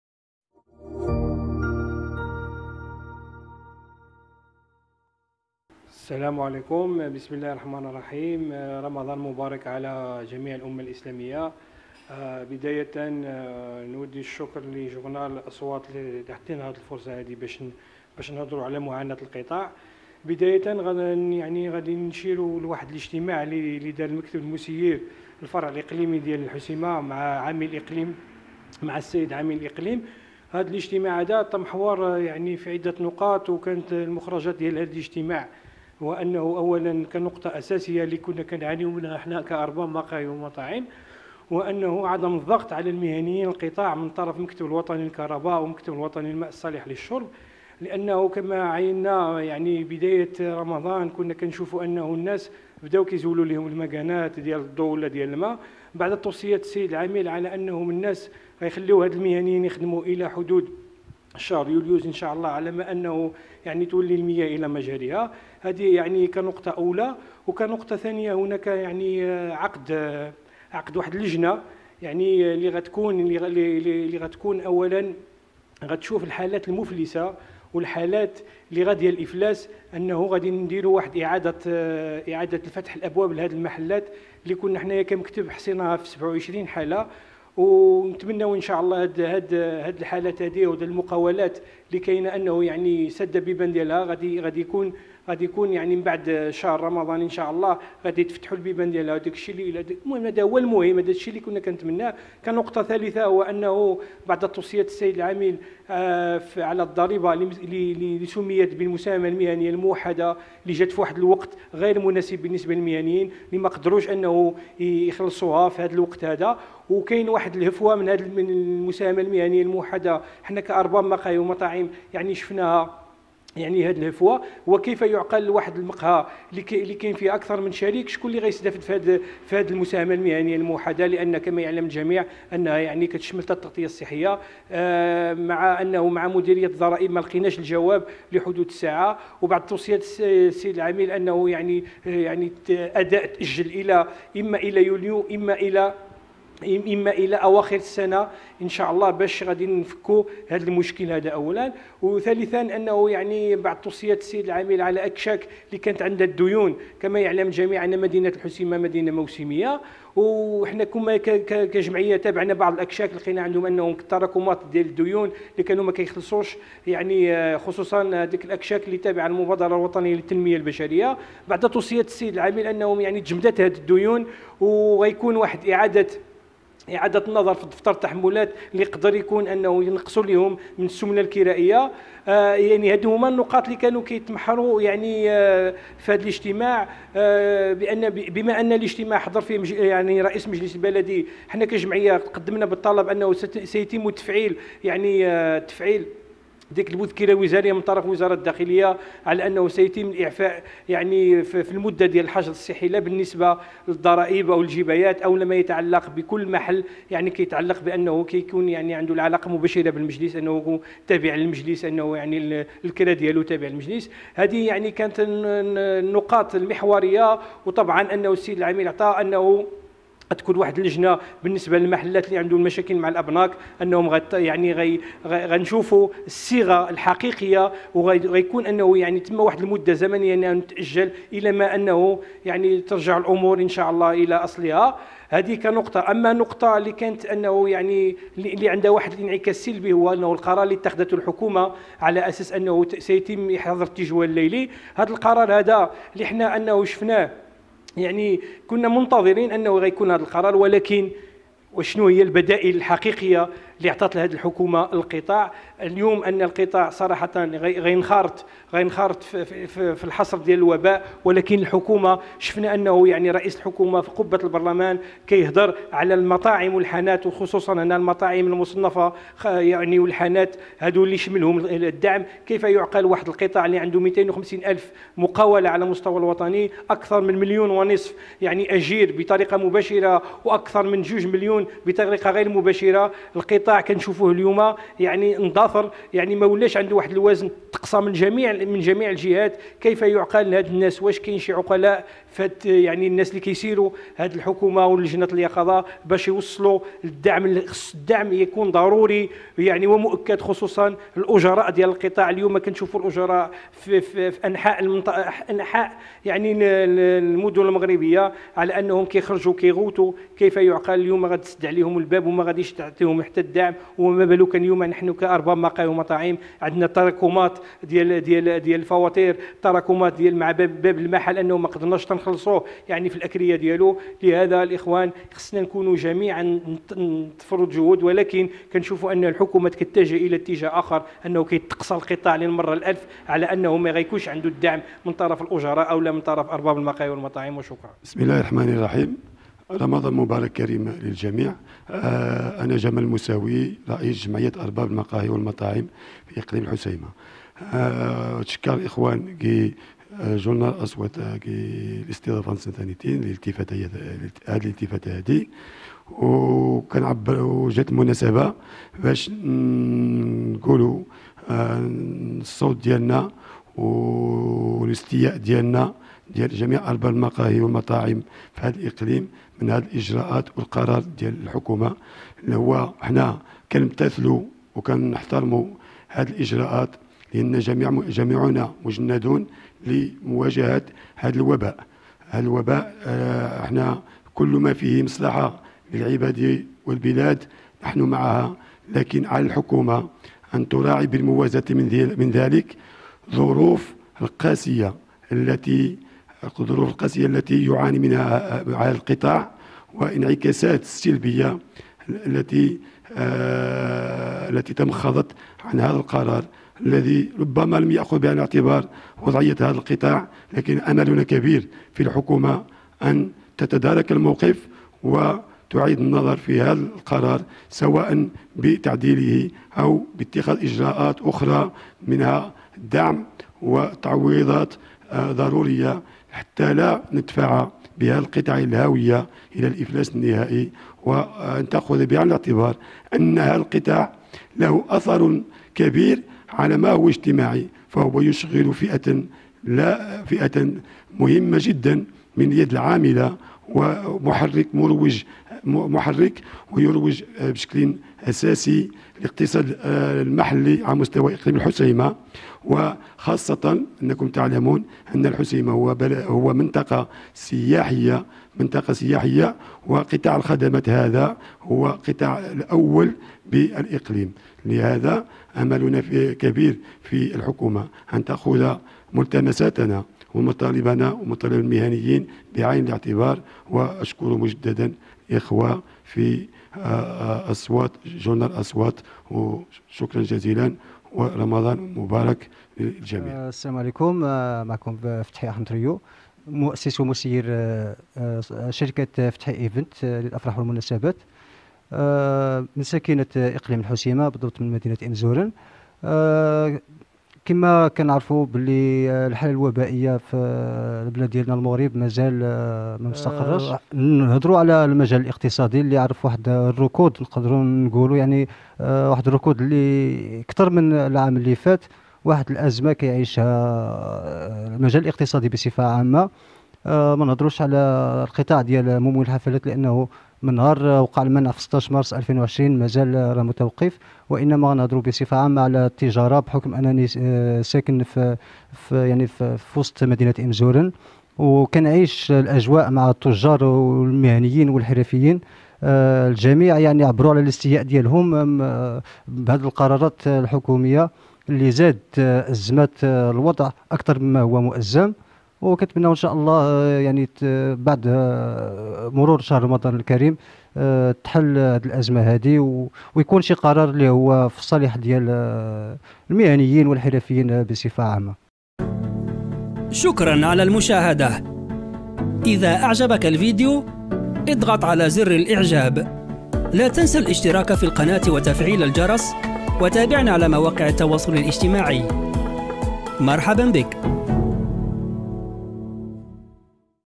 هذه هي مطالب قطاع المقاهي والمطاعم التي استجاب لها عامل إقليم الحسيمة
2. اضغط على الجرس (🔔) بجانب زر الاشتراك
3. شارك مقاطع الفيديو مع أصدقائك وعائلتك
    يشرفنا اشتراكك بالقناة